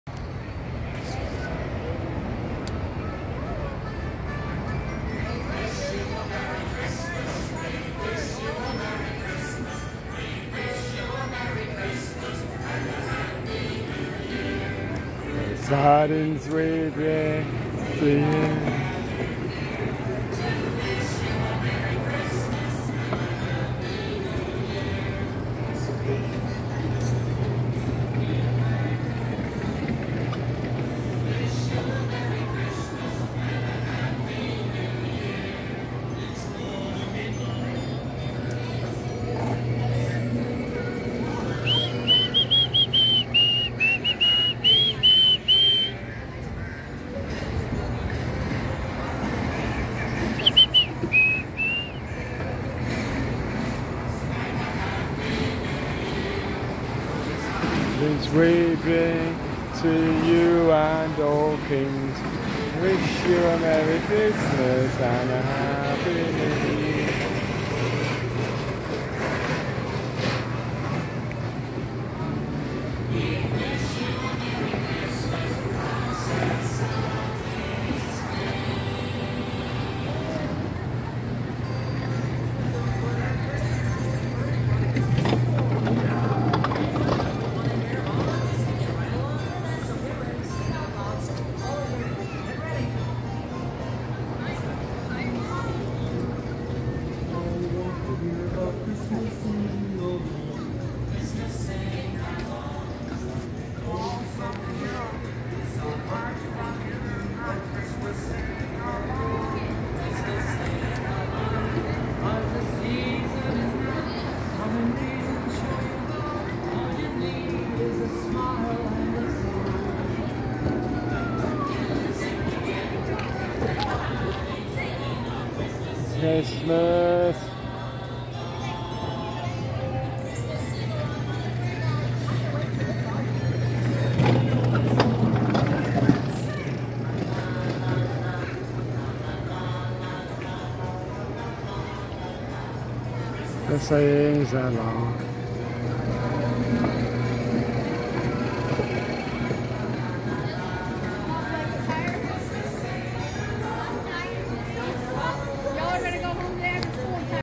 Sesame Street Car Ride. Busch Gardens Tampa Bay USA. Filmed November 2023.
The Sesame Street Car ride at Busch Gardens Tampa Bay USA. Off Ride Footage.

Attractions all around
Lee d holden